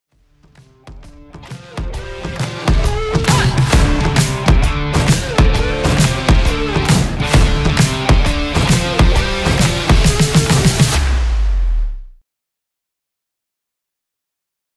Pantalons de travail - Würth MODYF
Découvrez la collection de pantalons de travail Würth MODYF, où style & confort rencontrent fiabilité et sécurité pour tous les corps de métiers.